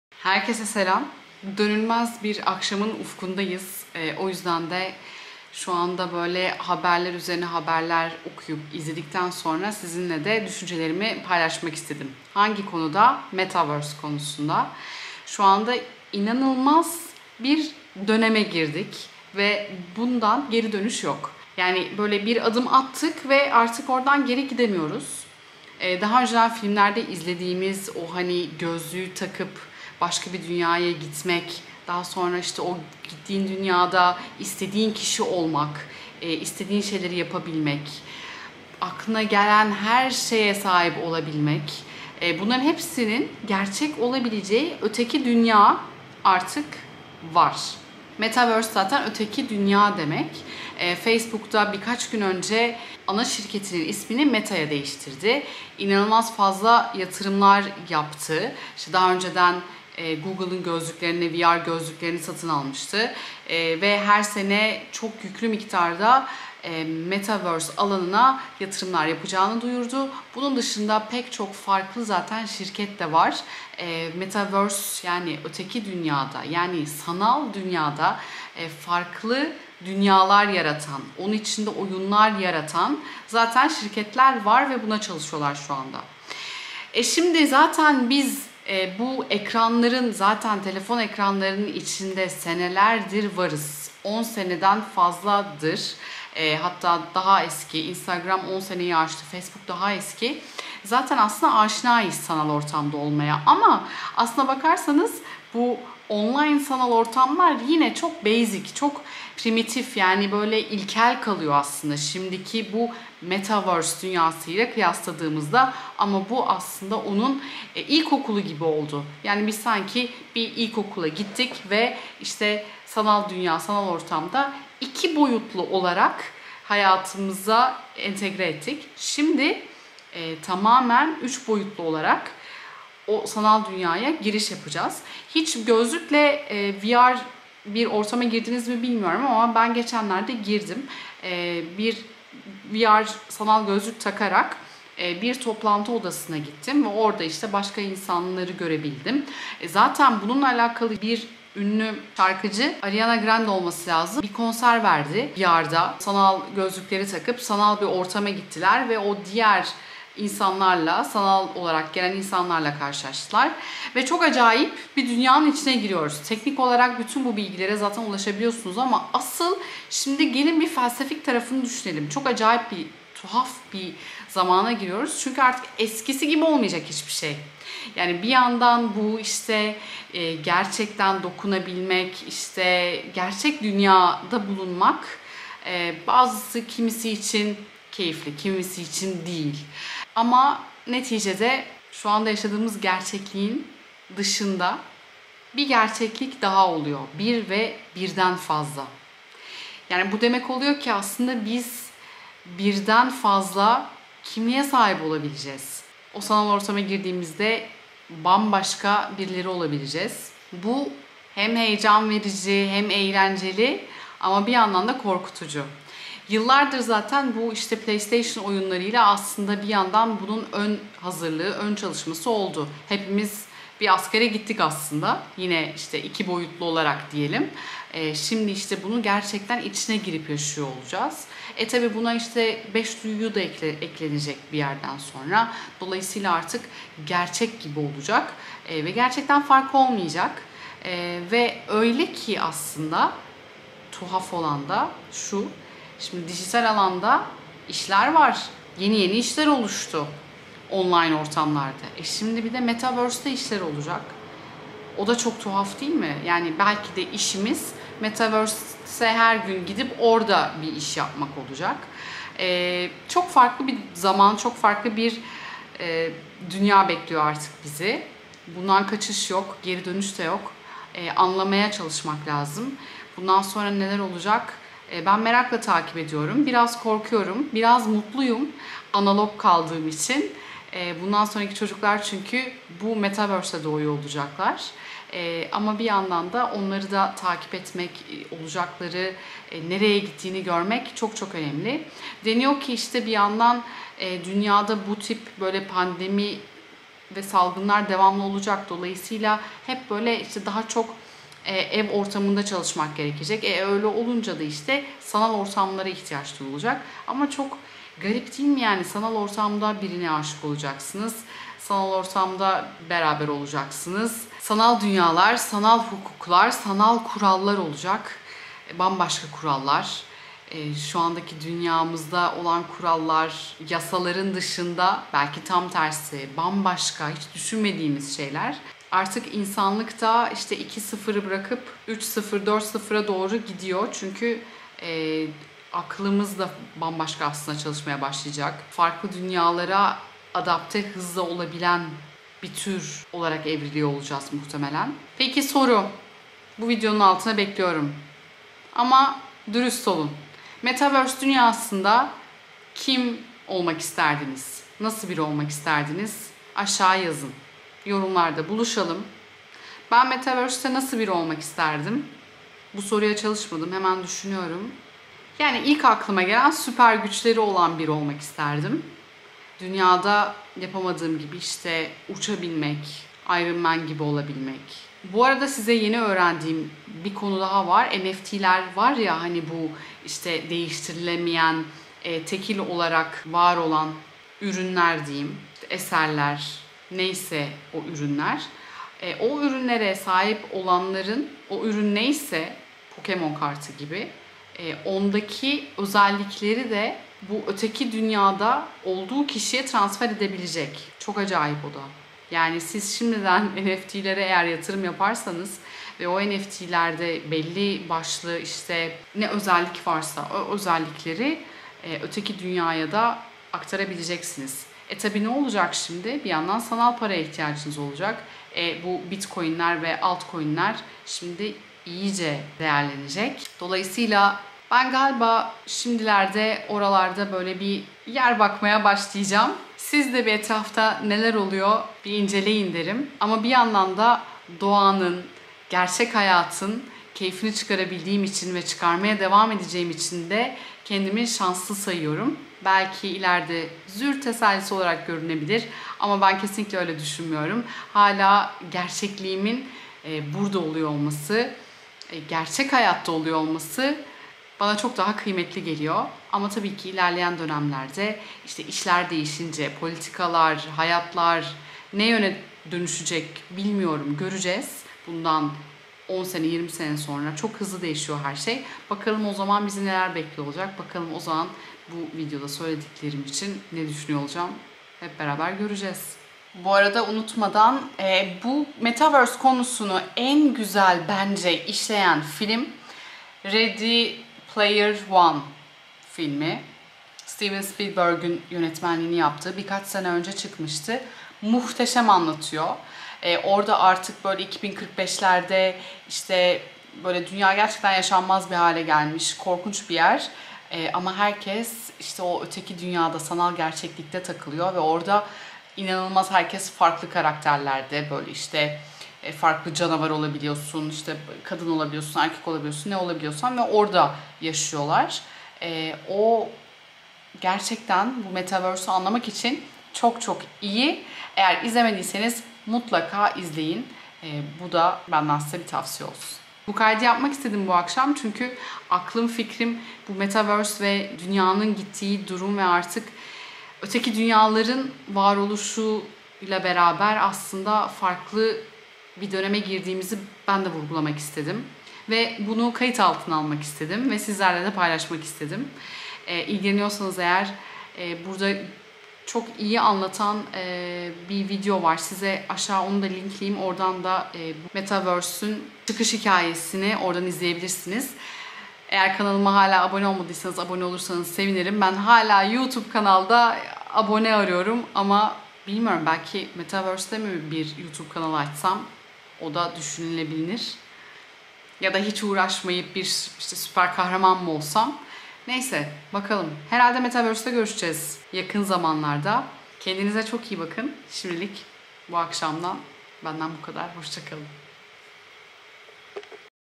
NEDİR BU METAVERSE? | Öteki Dünya'da Bizi Neler Bekliyor?
Metaverse nedir? Sanal bir öteki dünyalar kuruluyor ve biz buna hazır mıyız? Bizi neler bekliyor?
Biraz değerlendirelim.